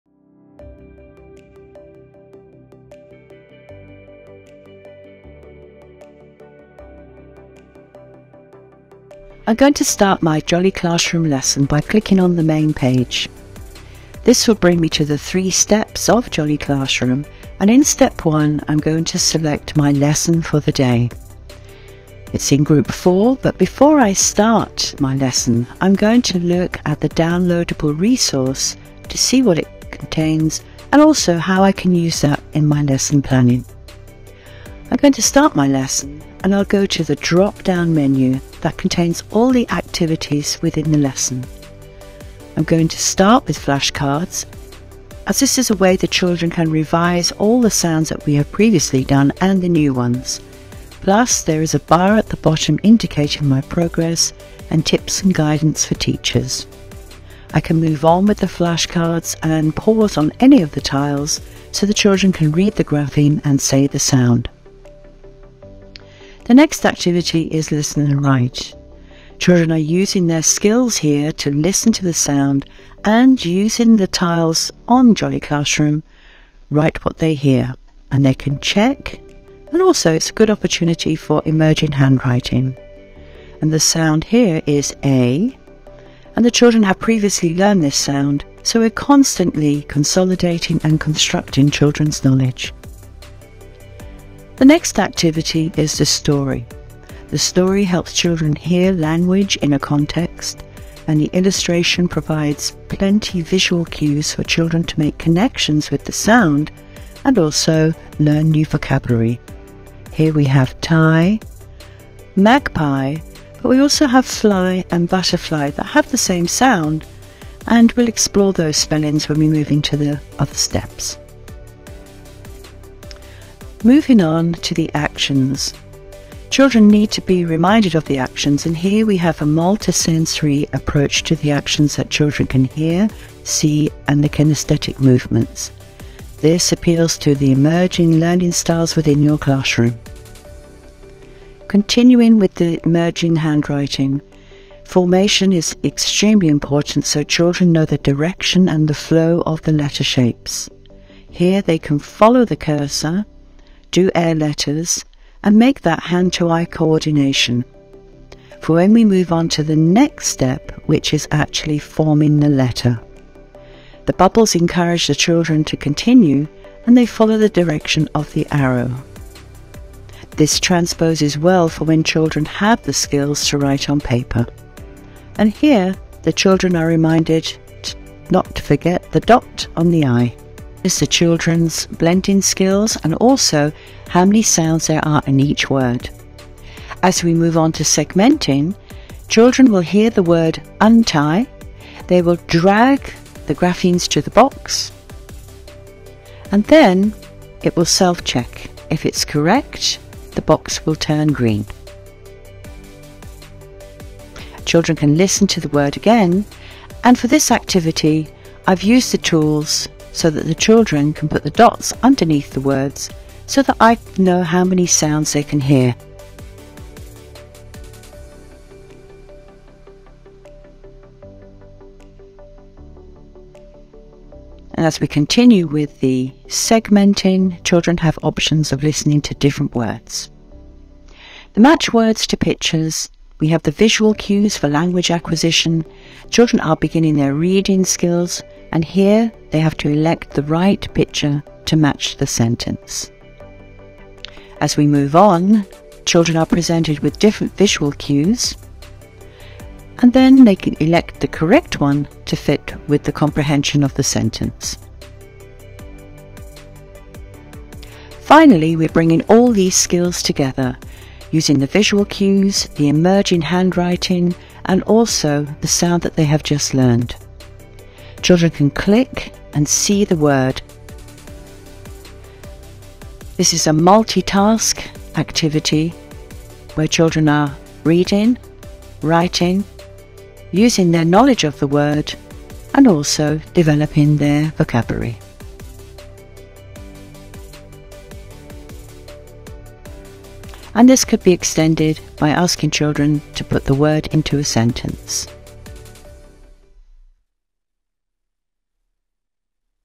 Jolly Classroom: Step 1 Walkthrough
How to set up Jolly Classroom and edit basic settings.